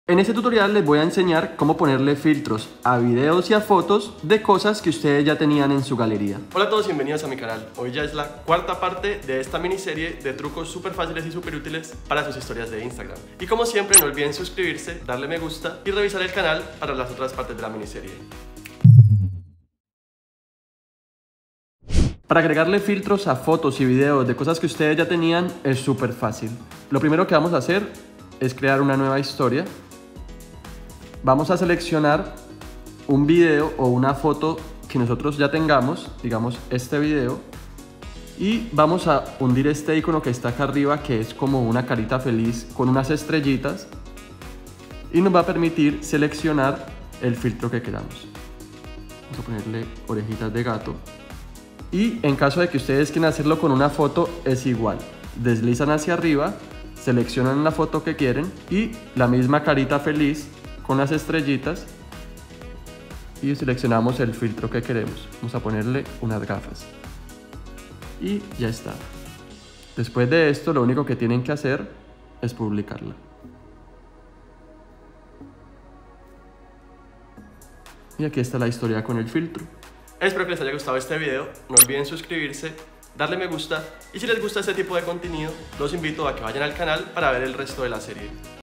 Como activar los nuevos FILTROS de Instagram 2019
Para activar los nuevos filtros a tus historias de Instagram 2019 con fotos o videos que ya tenias en la galeria es muy sencillo.

Esta es la 4ta parte de una mini serie de trucos super faciles para tus historias de Instagram 2019.
Lo primero que debes hacer es oprimir el boton para crear una nueva historia, despues buscas en tu galeria (ya sea seleccionando la el icono de galeria de fotos o deslizando hacia arriba la pantalla) selccionas la imagen y el video que deseas, lo posicionas.
El segundo paso es oprimir el icono en forma de cara feliz con estrellas que esta en la parte de arriba de la pantalla y ahi estan todos los filtros que puedes seleccionar , recuerda que este truco funciona en videos de menos de 15 segundos y en cualquier foto donde se pueda detectar el rostro.